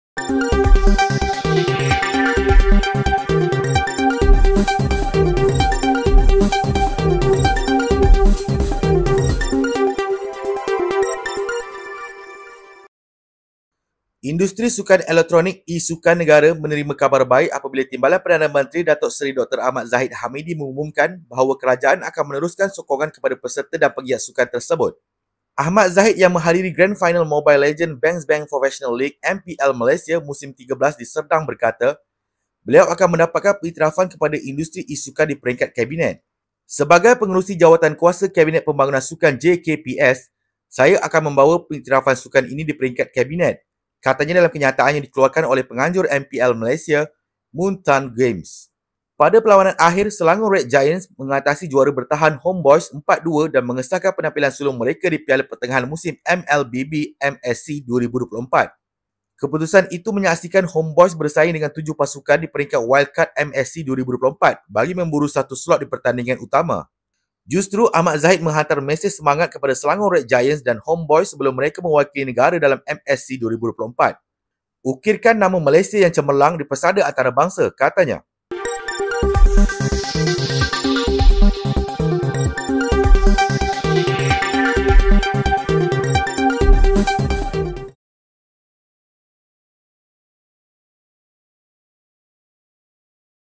04.06.2024 - MADANI TERKINI | E-SUKAN NEGARA TERIMA SOKONGAN KERAJAAN
Industri sukan elektronik (e-sukan) negara menerima khabar baik apabila Timbalan Perdana Menteri, Dato’ Seri Dr. Ahmad Zahid Hamidi mengumumkan bahawa kerajaan akan meneruskan sokongan kepada peserta dan penggiat sukan tersebut.